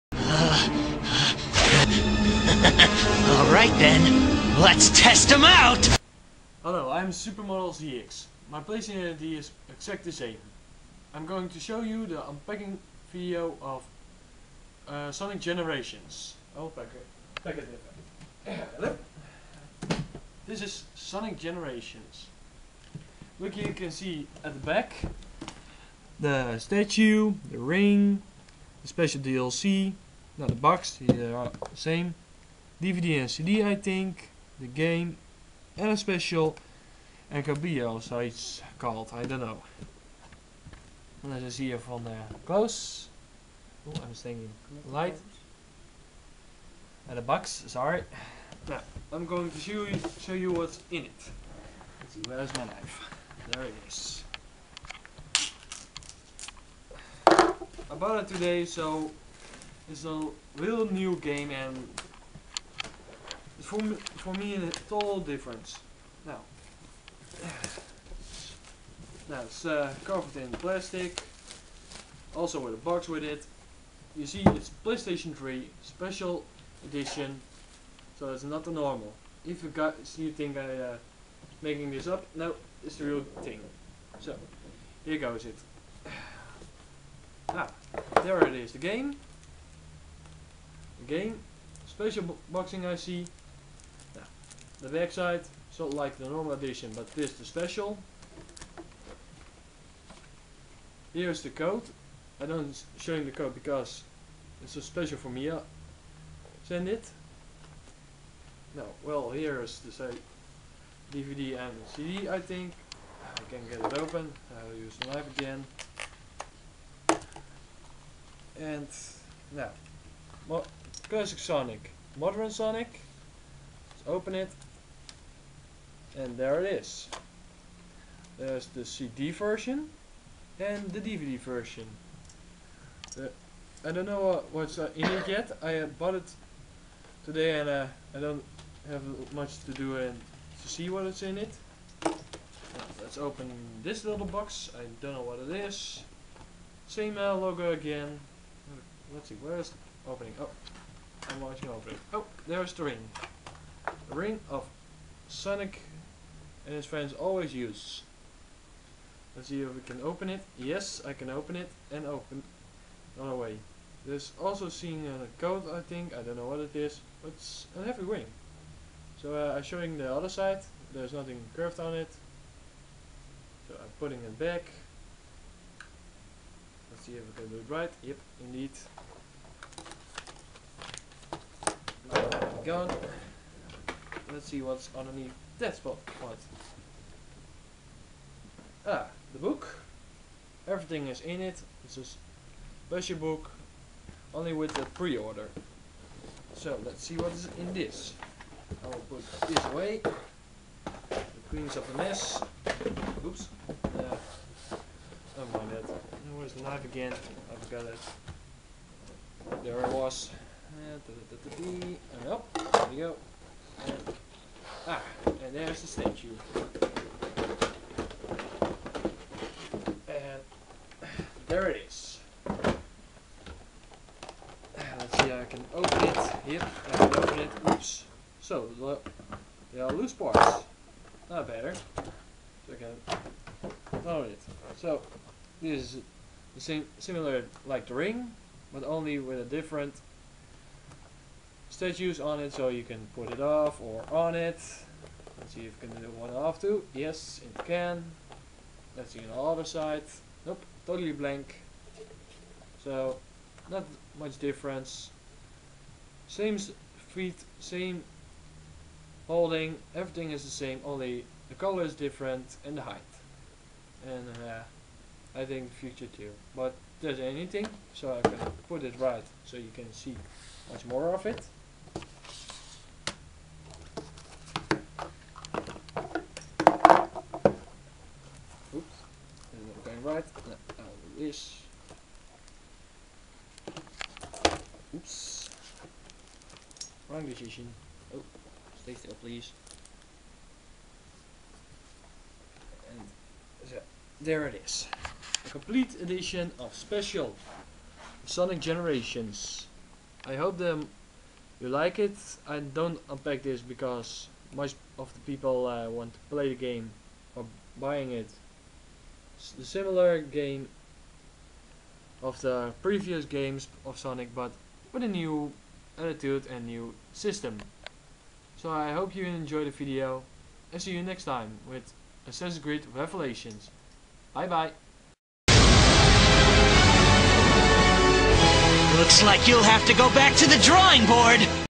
SuperModelZX Presents Unboxing Sonic Generations
This is A Unboxing video of Sonic Generations

Made By: Me (SuperModelZX)

The Video was made by a Good Friend of mine. I only did the talking and the rest what you see in the video. It's really me